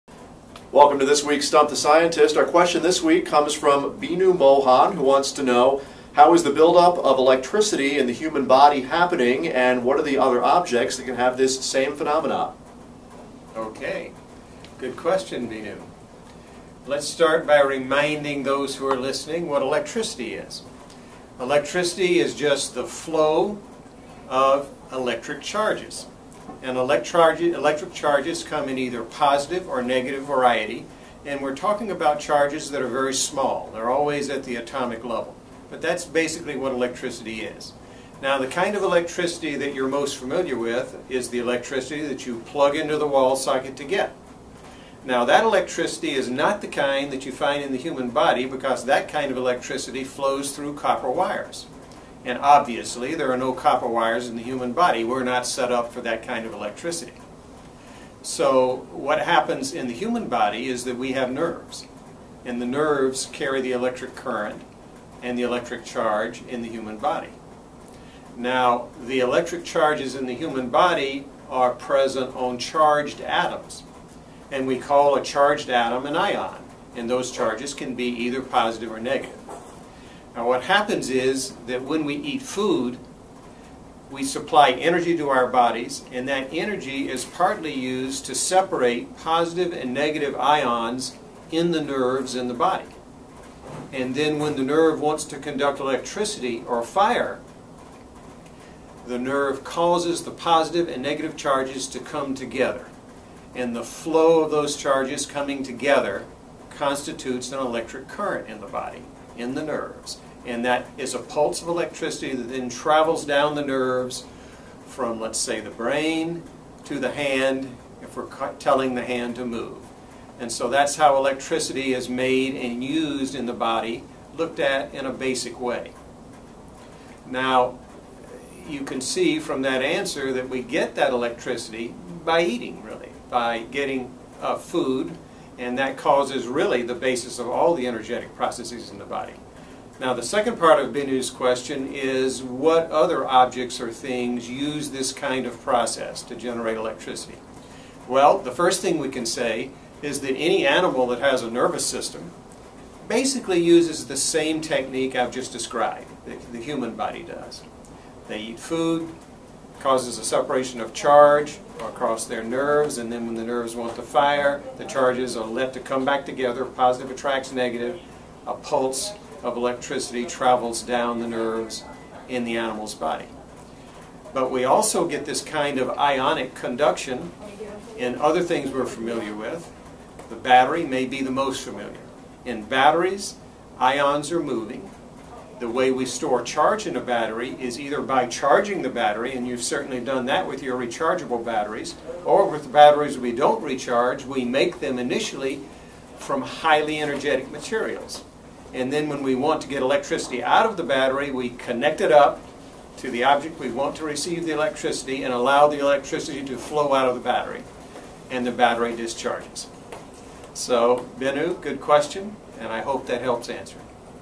Stump the Scientist: Electricity in the Human Body
The GE scientists are taking on your toughest science questions in our "Stump the Scientist" series! and challenging our scientists to bring you the answers. This week's question: How is the build up of electricity in human body happening and what are the other objects that can have this same phenomena?